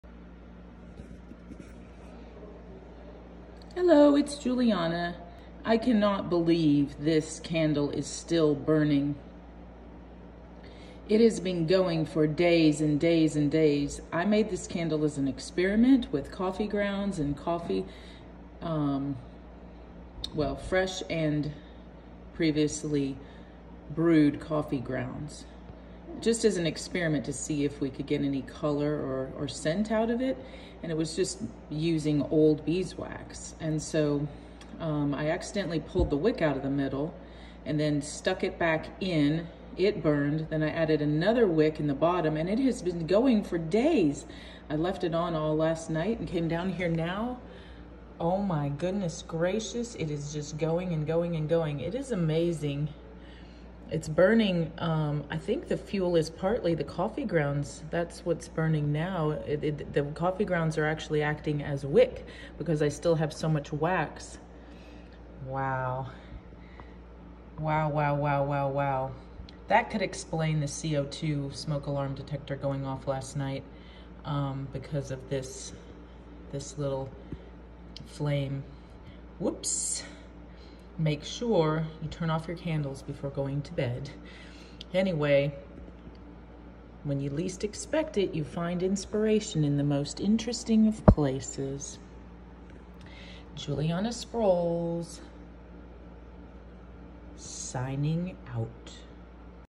The Little Candle That Could
I made this as a tester candle with coffee grounds and old beeswax from previously burned candles ✨ the wick detached so I simply put it back in and later added a second one ✨ I have discovered now the coffee ground are acting as wick! It gas been burning for days 🔥

This is the little candle that could 😀

Shine Bright ✨🌈
Be Well ☀️